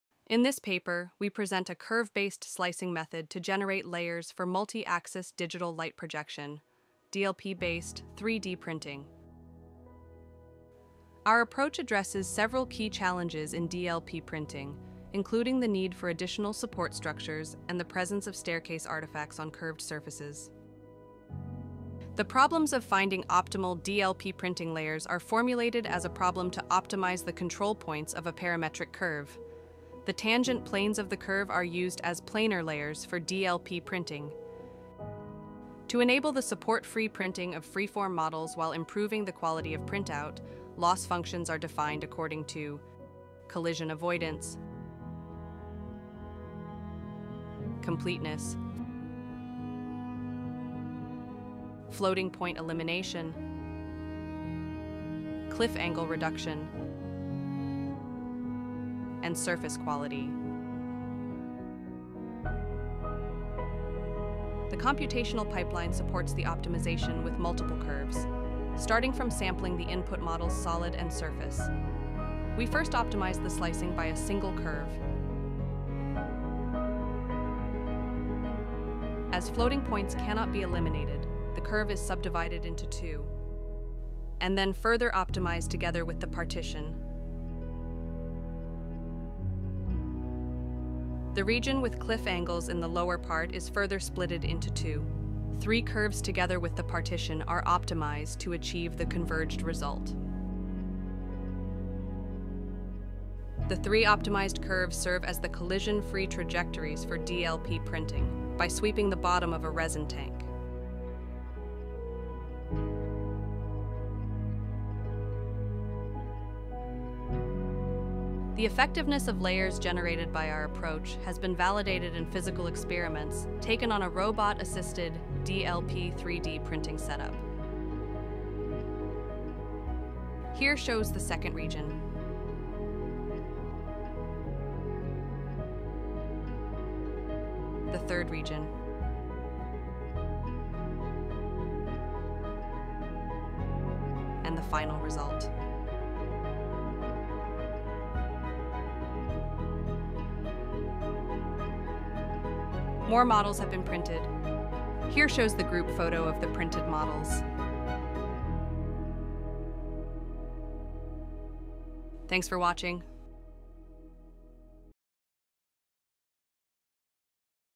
Curve-Based Slicer for Multi-Axis DLP 3D Printing | SIGGRAPH Asia 2025
In this work, we introduce a novel curve-based slicing method for generating planar layers with dynamically varying orientations in digital light processing (DLP) 3D printing. Our approach effectively addresses key challenges in DLP printing, such as regions with large overhangs and staircase artifacts, while preserving its intrinsic advantages of high resolution and fast printing speeds. Our method has been validated through physical experiments on a robotic multi-axis DLP printing setup, demonstrating that the optimized curves can robustly guide smooth, high-quality fabrication of complex geometries.

This research work has been accepted to publish in the following paper:
Chengkai Dai, Tao Liu, Dezhao Guo, Binzhi Sun, Guoxin Fang, Yeung Yam, and Charlie C.L. Wang, "Curve-based slicer for multi-axis DLP 3D printing", ACM Transactions on Graphics (SIGGRAPH Asia 2025), vol.44, no.6, December 2025.

(Source code will be released after December 2025)